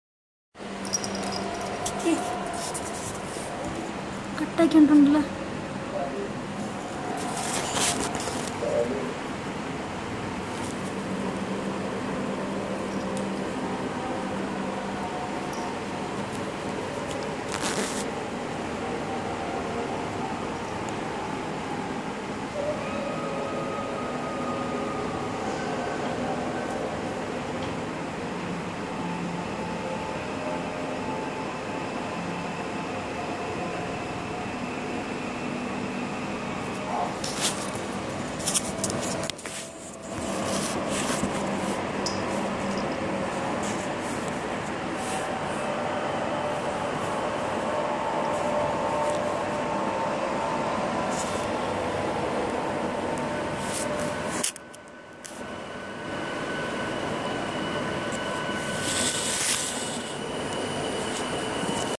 Baby's first catnip reaction,Catnip stuffed in toy
Catnip leaves stuffed in soft mouse toy